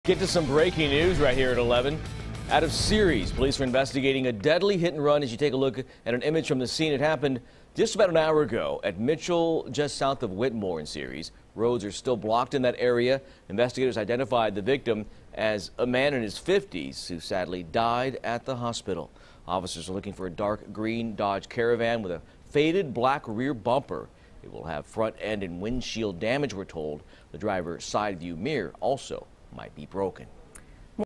Ceres Police Investigating Deadly Hit And Run
Police say a man is dead after a hit-and-run crash in Ceres Tuesday night.